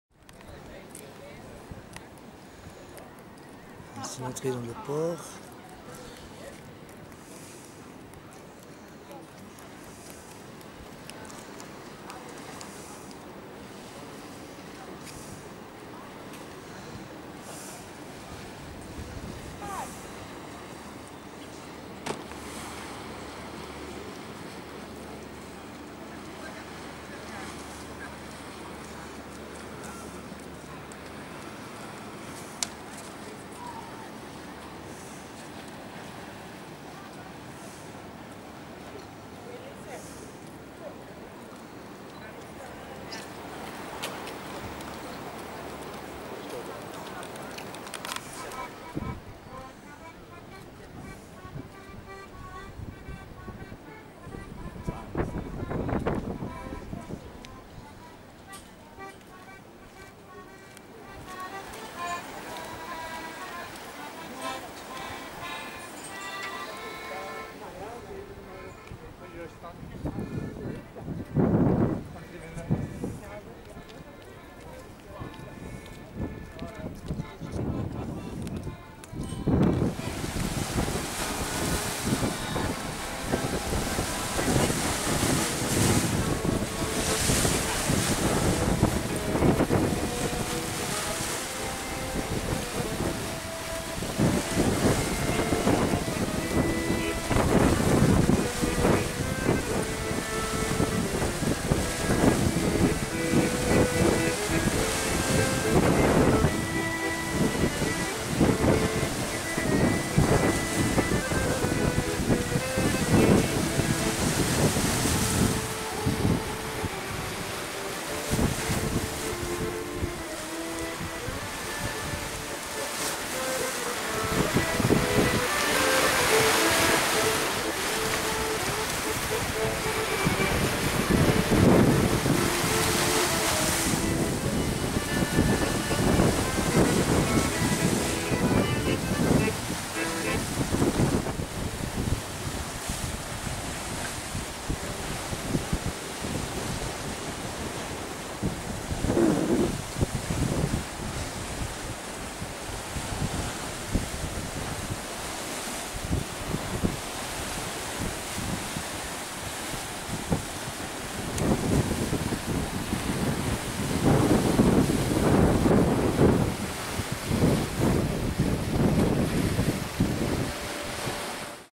chania crete
jolie ville  cette  sité de  chania sur l'île de  crete ou je susi allé pour une journée il yavait pas  mal de  vent  mais le temps est superbe